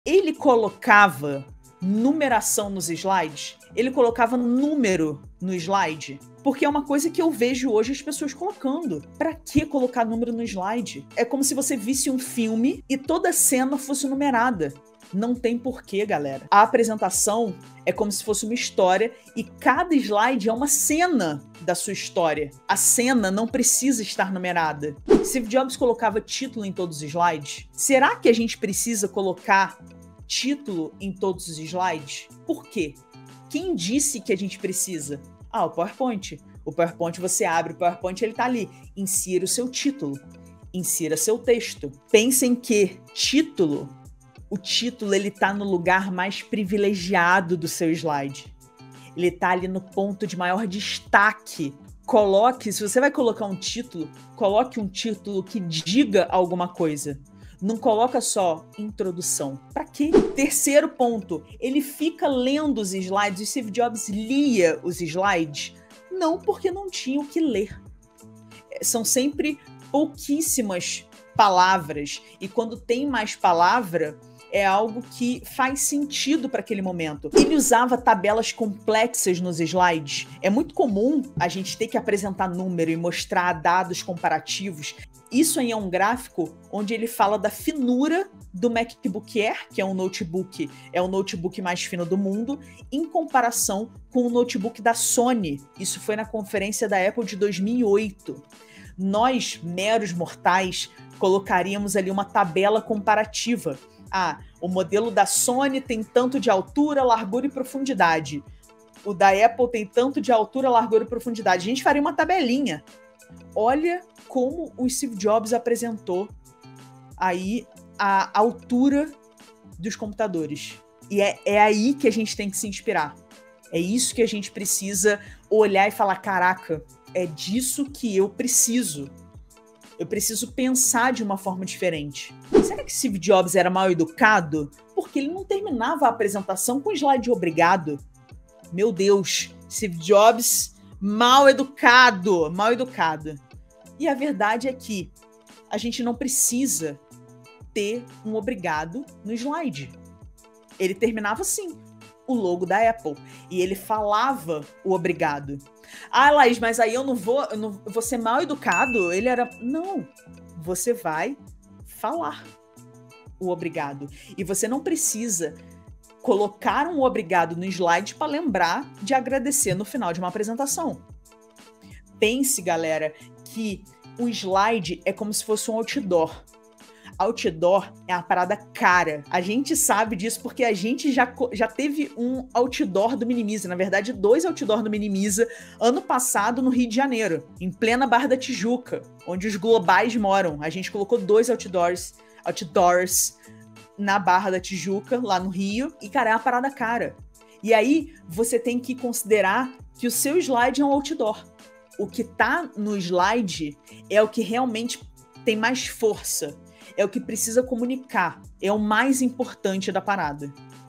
5 COISAS QUE O STEVE JOBS NUNCA FEZ EM SUAS APRESENTAÇÕES
Olá, PPTeiro! O Steve Jobs era um gênio da comunicação que, de forma simples, impressionava a todos com as suas apresentações.